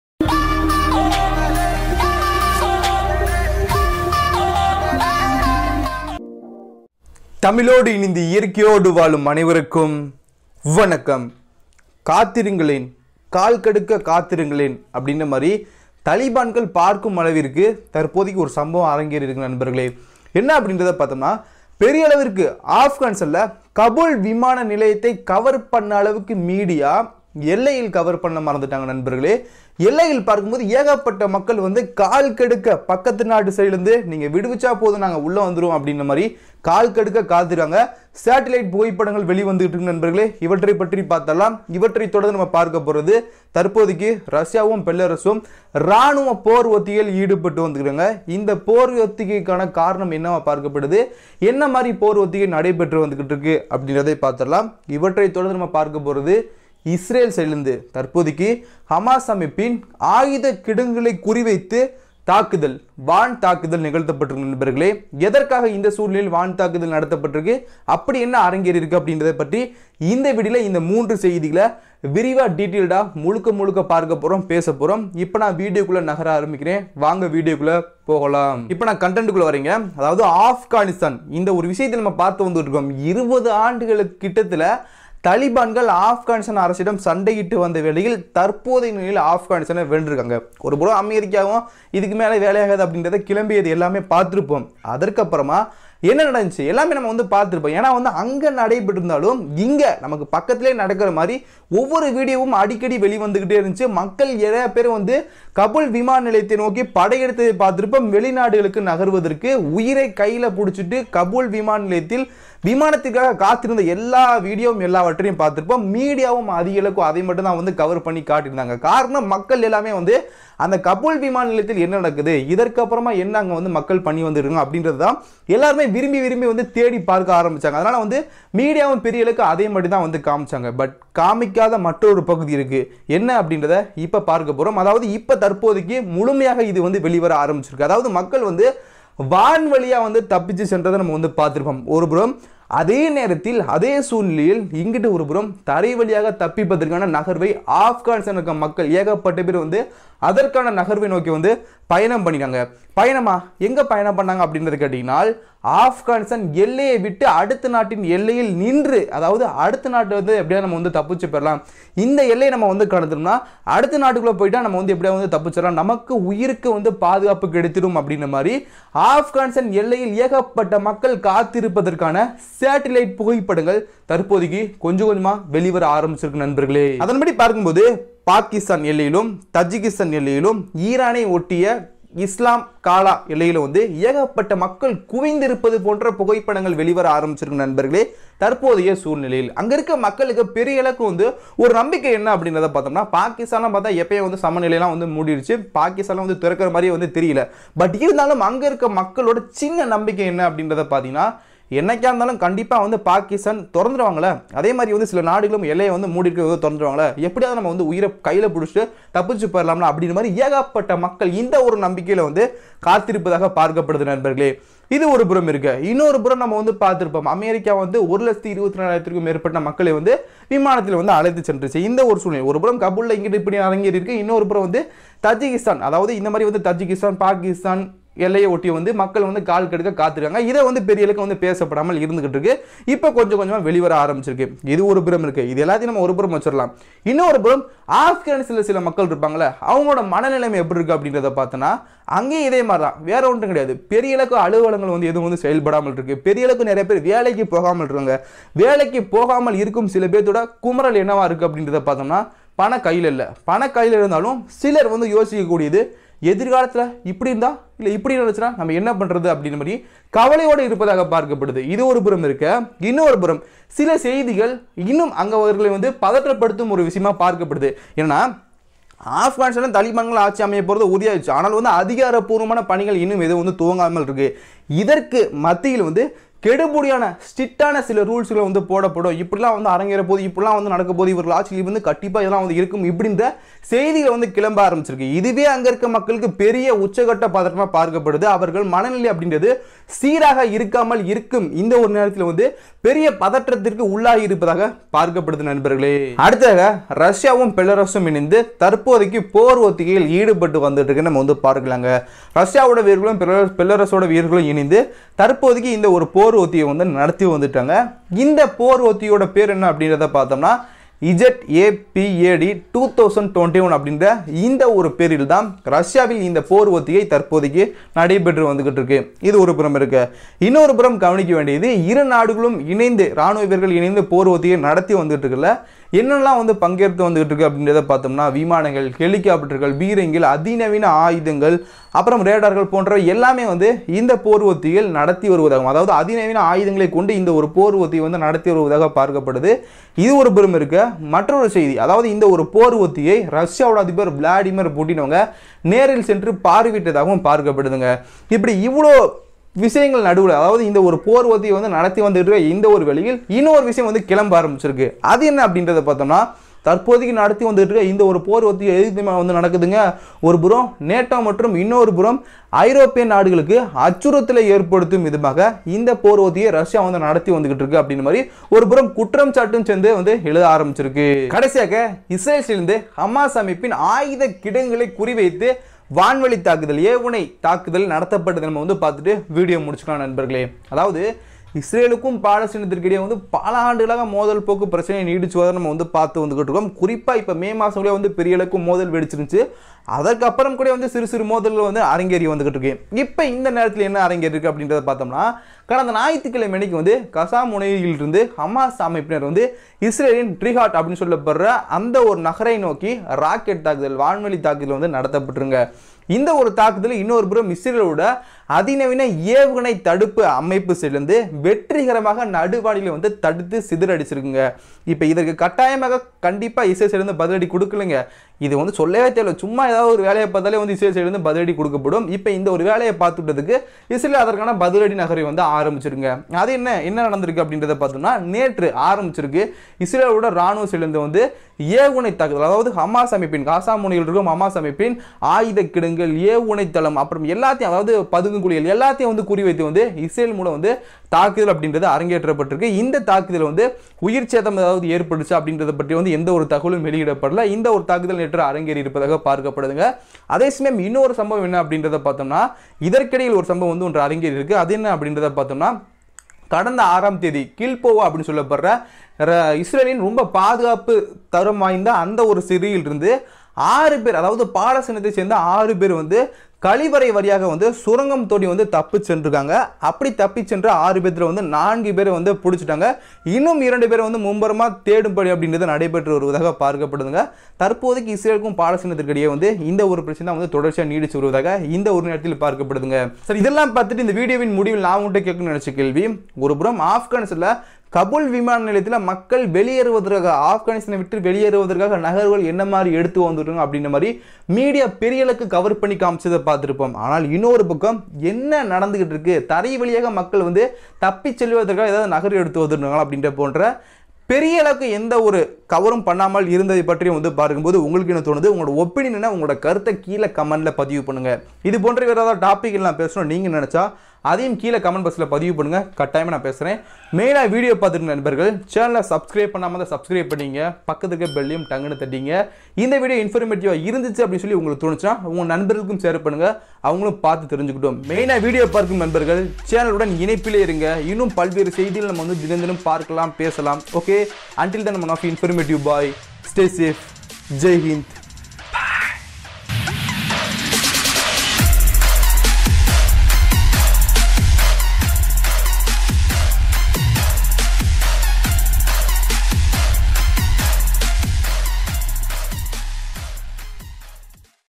என்ன கொடுமடா இது | what is happening in Afghanistan right now | Tamil | INFORMATIVE BOY
Education, information, Trending news, News, Strory, Current Affairs